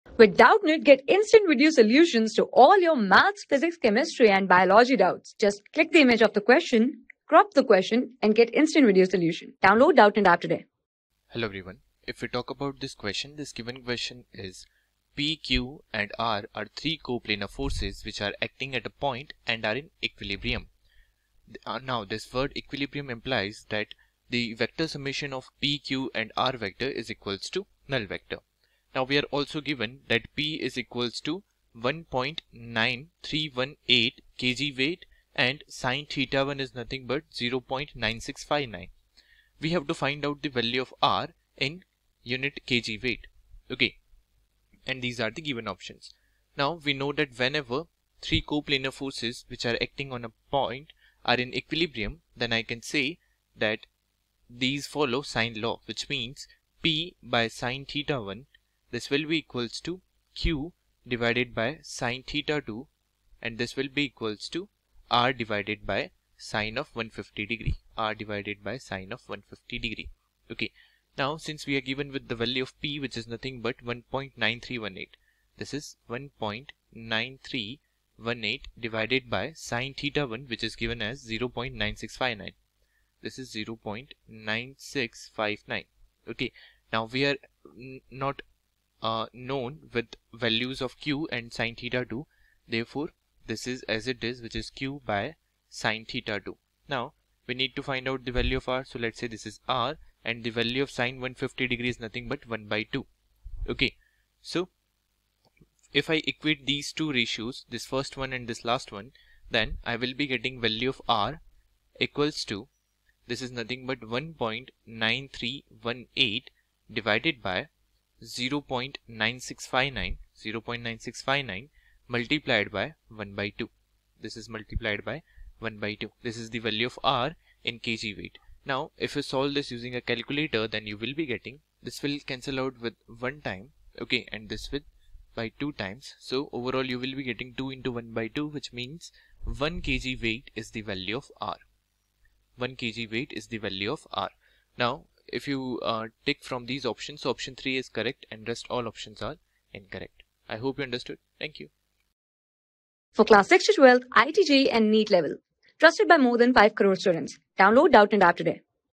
P, Q and R are three coplanar forces acting at a point and are in equilibrium. Given P= 1.9318 k...
P, Q and R are three coplanar forces acting at a point and are in equilibrium. Given P= 1.9318 kg-wt, sin theta_(1)= 0.9659, the value of R is (in kg-wt)
Class: 12
Subject: PHYSICS
Chapter: BASIC MATHEMATICS & VECTORS
Board:IIT JEE

👉 Have Any Query? Ask Us.